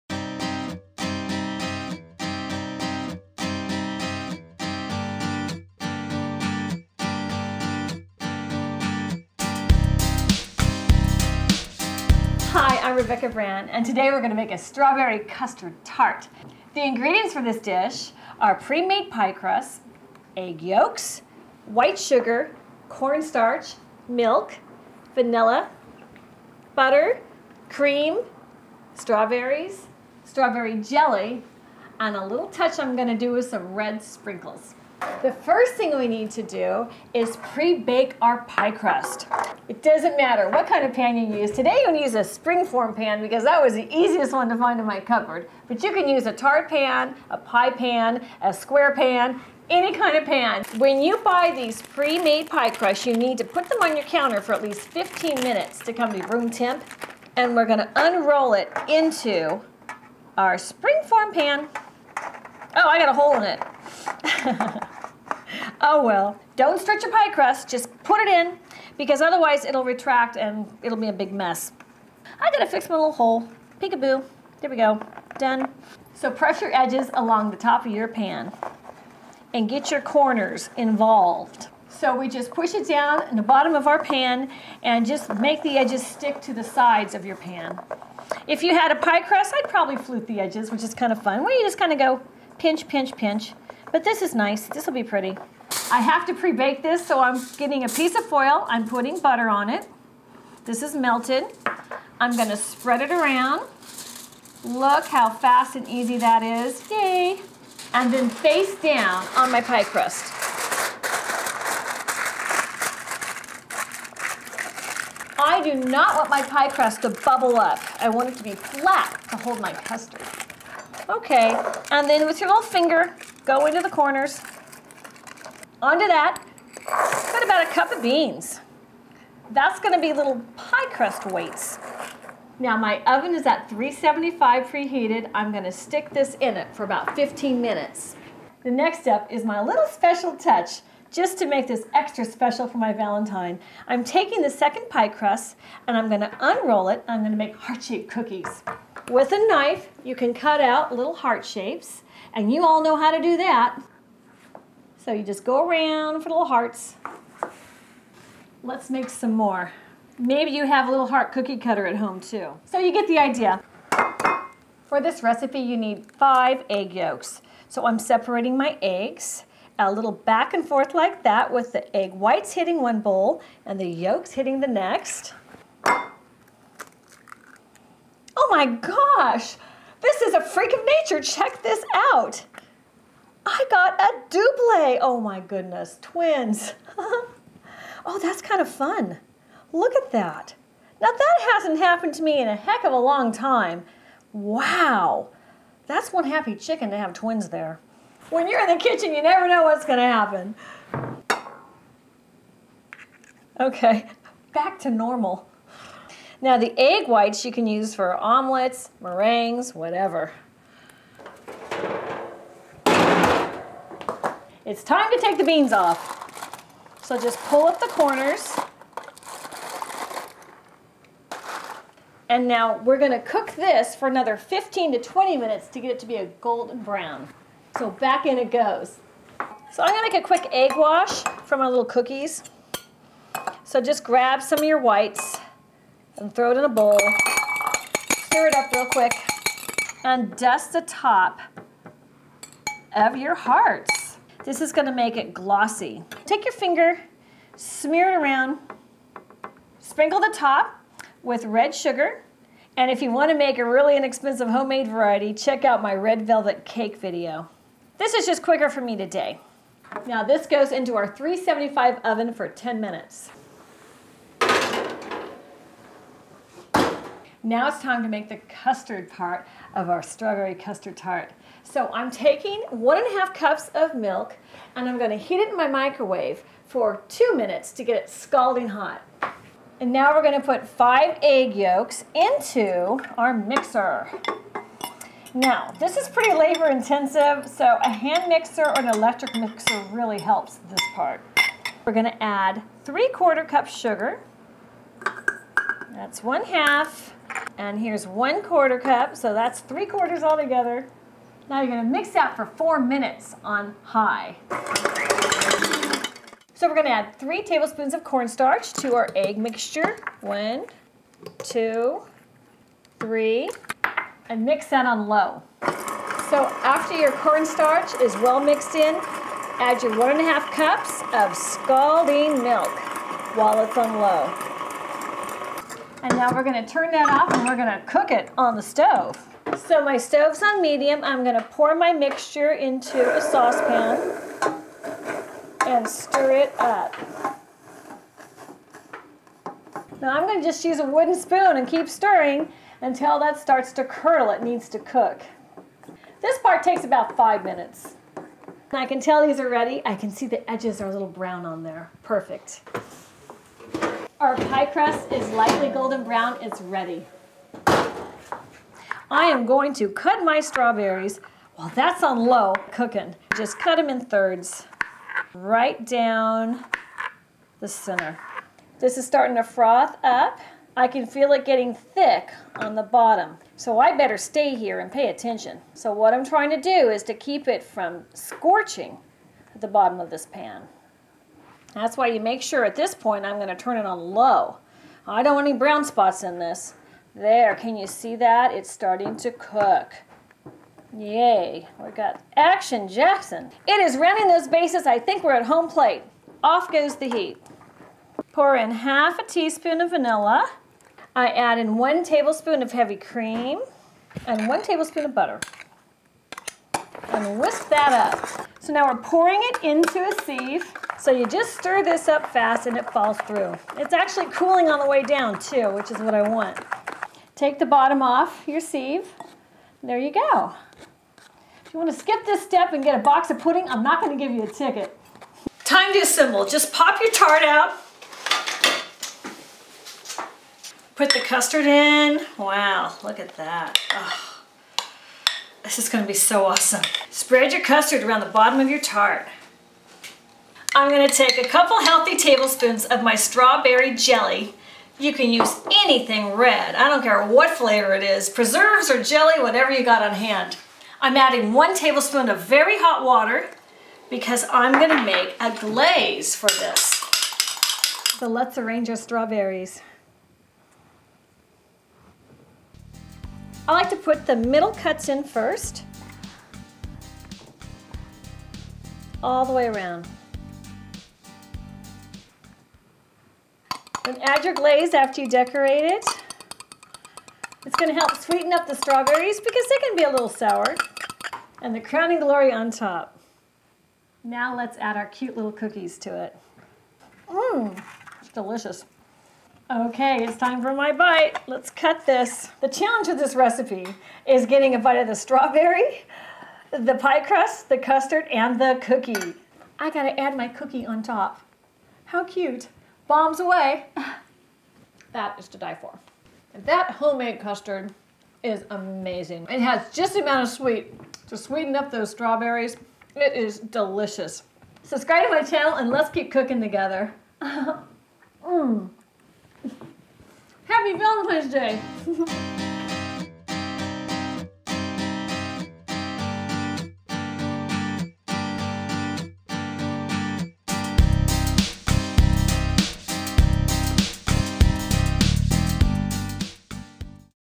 Strawberry Tart Recipe : How to make Strawberry Tart
Learn Strawberry Tart Recipe : How to make Strawberry Tart
Rebecca Brand shows how to make a classic custard fruit tart with strawberries.  Homemade vanilla custard, the very classic French custard way, holds the strawberries, the fruit, on top of a delicate tart crust.  Valentine heart cookies add fun to the dish and make it a special Valentine present your honey bunny.  Strawberry Tarts are a special Spring treat, and this Strawberry Custard Tart is the most amazing of desserts, yet Rebecca shows you how to do it easily simplifying the steps so your Strawberry Tart comes out perfect.  Rebecca's how to technique makes it so you can make this Strawberry Tart fast and beautiful.  Sugar cookies shaped like Valenitine Hearts add the special touch for your lovey dovey to feel appreciated and adored on Valentine's Day as a special addition to this recipe that's easy and inexpensive.  Homemade Strawberry Custard Tarts are the perfect Valentine dessert.